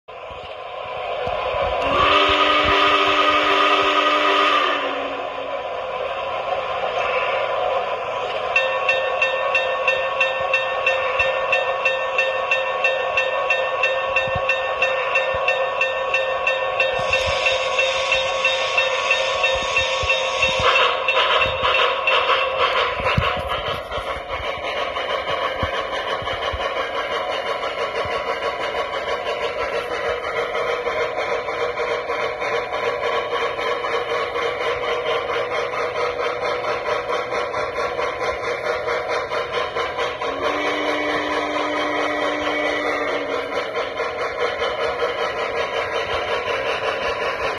Big One Model w/t sounds & steam (by Otaku Railfan)
Credits to Otaku Railfan; owner of this model, showcasing it for fun. Don't ask how I got this but either way it's best worth mentioning it.

He's good and the best Train Master Builder at it.